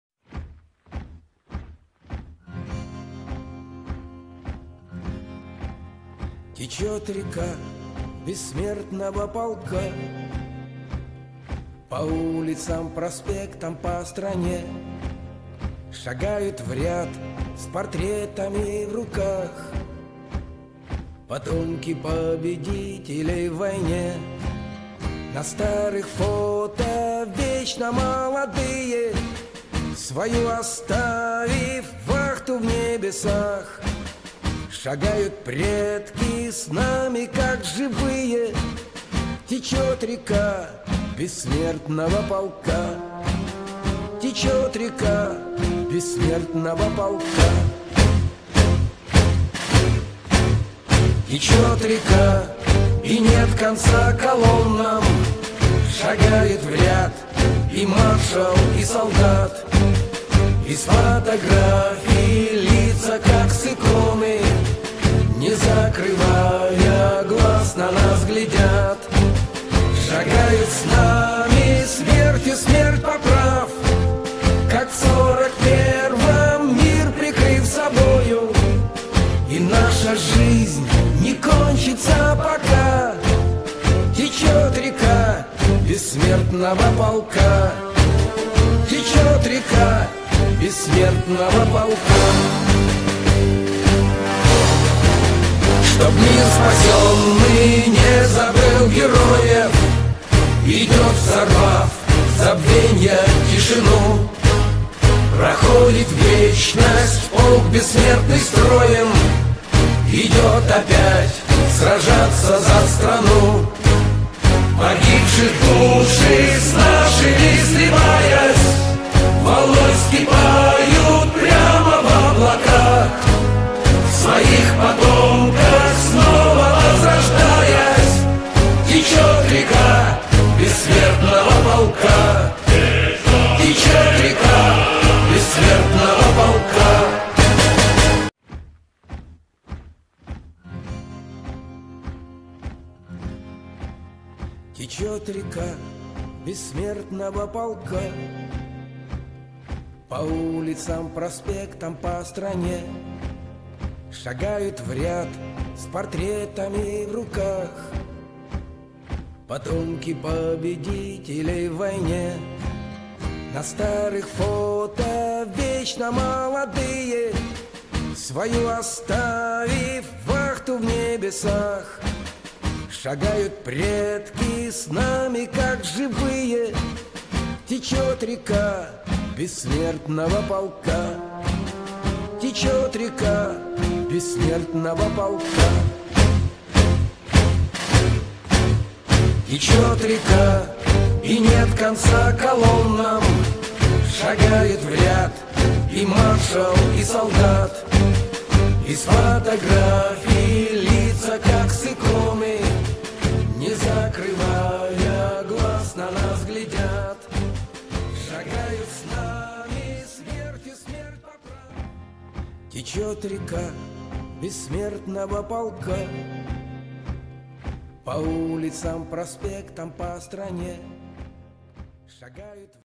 Қобда ауданы. "Мəңгілік даңқ"- "Бессмертный полк"
Қобда аудандық мәдениет және тілдерді дамыту бөлімі мен Мәденит үйі баршаңызды 9 мамыр – Жеңіс күнімен құттықтайды. Әлия Молдағұлова бастаған Кеңес Одағының 8 батыры мен Ұлы Отан соғысының ардагерлері және тыл еңбеккерлерінің суреттерін «Мәңгілік даңқ» акциясы аясында көрсетіп, олардың Жеңіс жалауы желбіреген күнді жақындатқаны үшін, жанқиярлық ерліктері мен еңселі еңбектеріне бас иіп, тағзым етеді!
Бессмертный полк